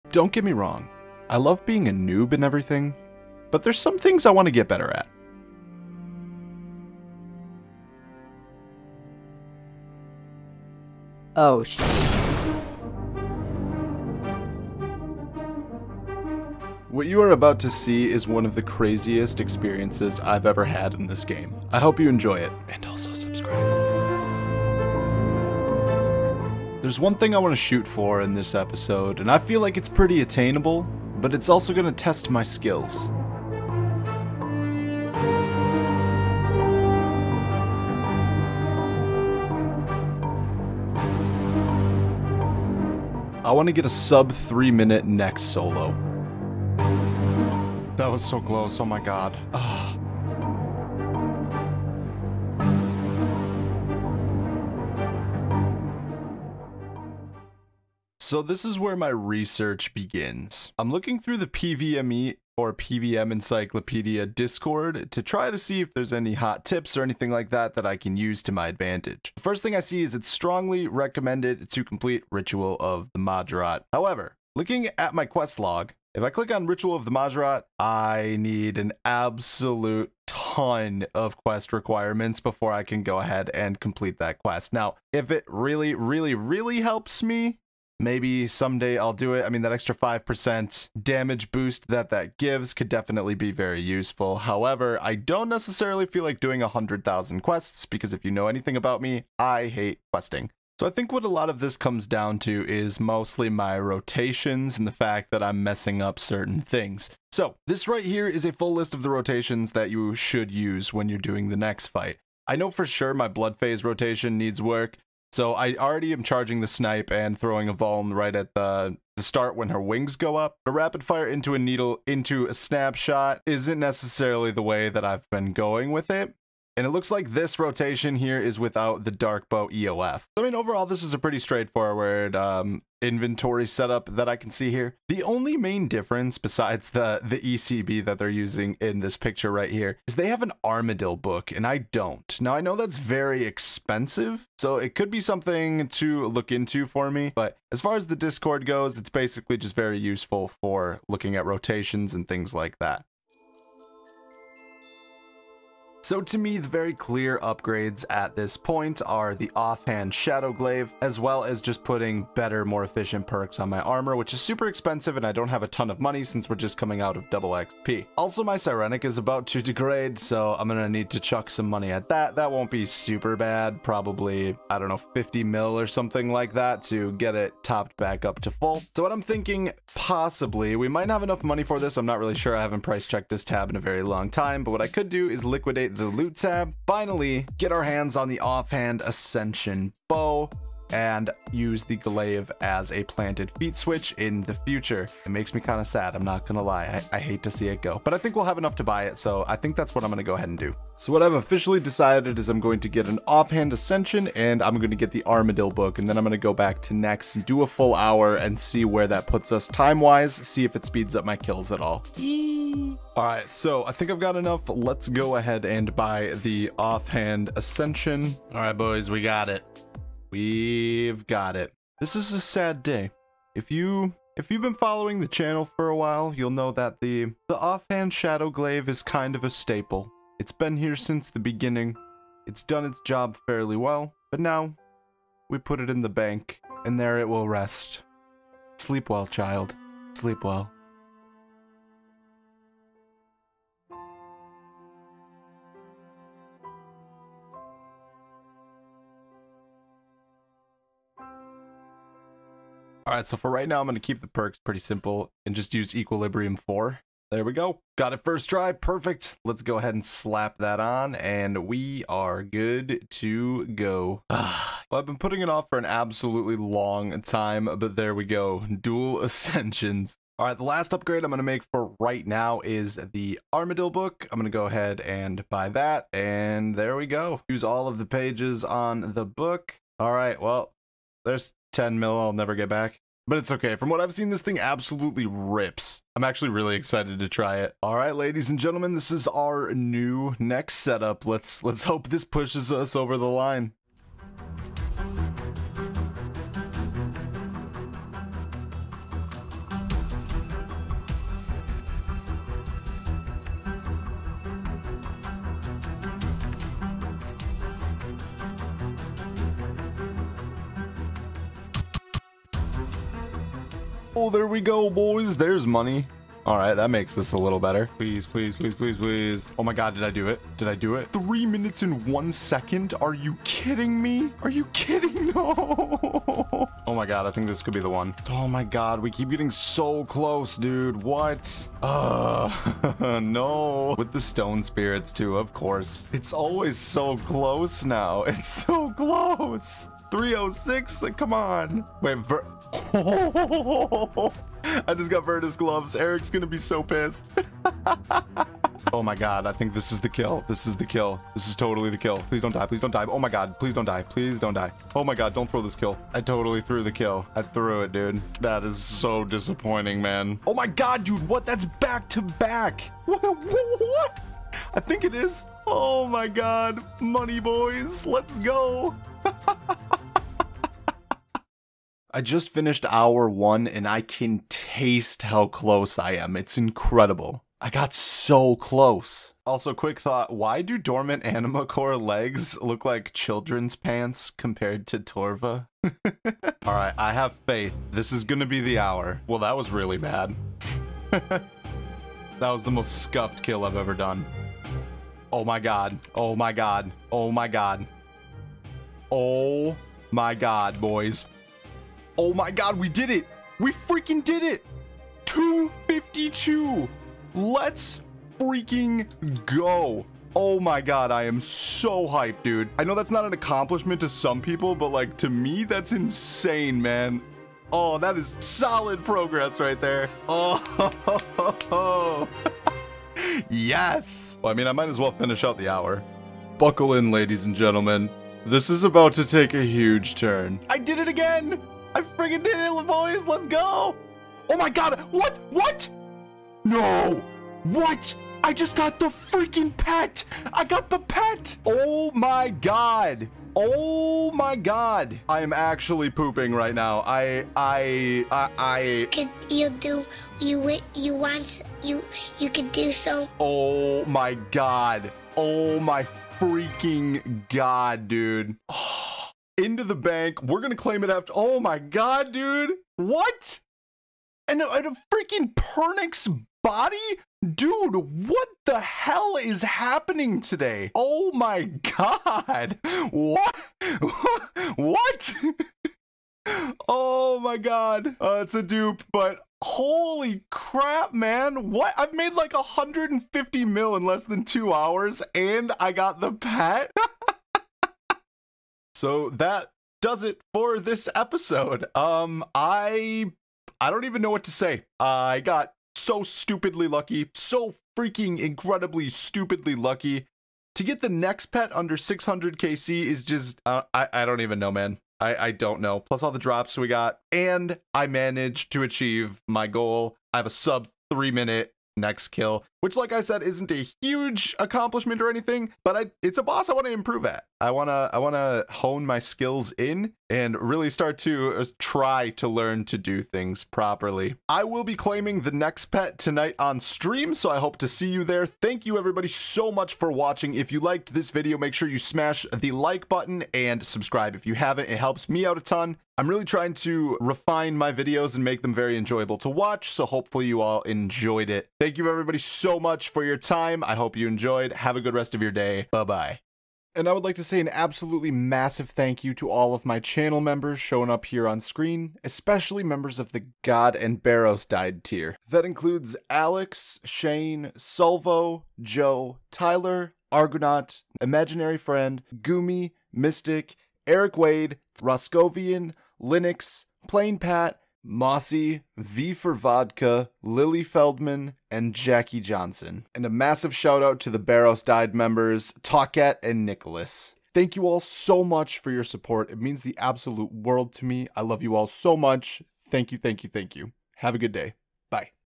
The Best Pet Unlocked! My Nex Luck Is Insane! - A Day Of Casual Money Making #17
This is my journey to obtain a Nex solo in under 3 minutes. I can definitively say that this is the luckiest experience I've had in game. I hope you enjoy :)